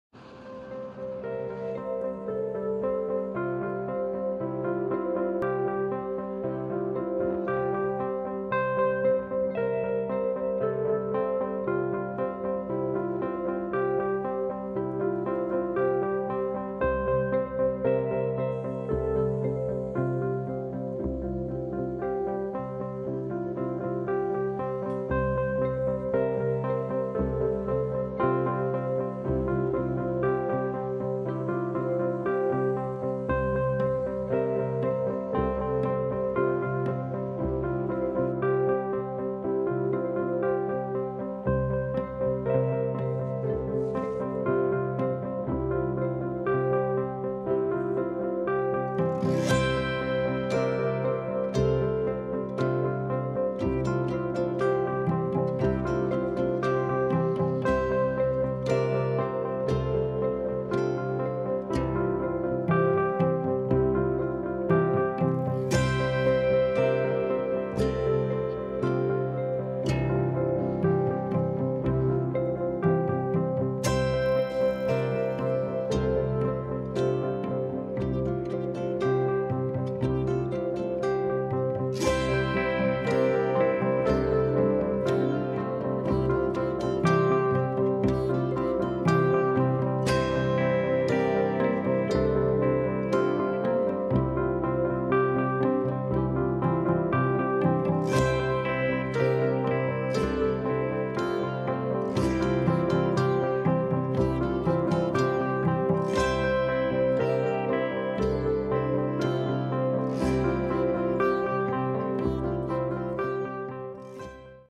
买菜也能赏“街景” 秋天的美-11月 11 多伦多Fall colours around me-November Toronto | howtoXiaoHui
买菜也能赏“街景” 秋天的美。shopping also enjoy the beauty of "street view" in autumn
请您欣赏买菜路上的秋色美！Beautiful fall colours.